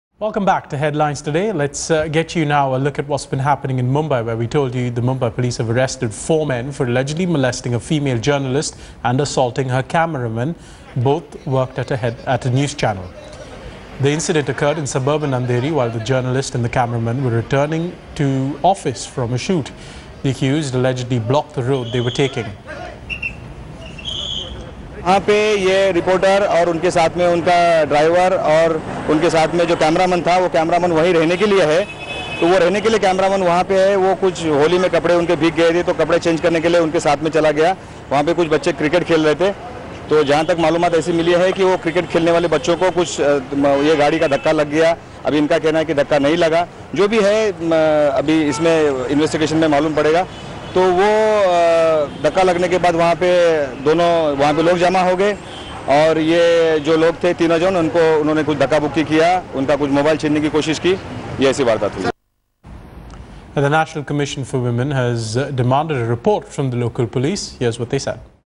Holi Horror: NCW demands report on journalist's molestation from cops
The National Commision for Women has asked the cops to take stringent action against those who have been caught for allegedly molesting a female journalist in Mumbai. They hav e also demanded to see the pertaining report on the case.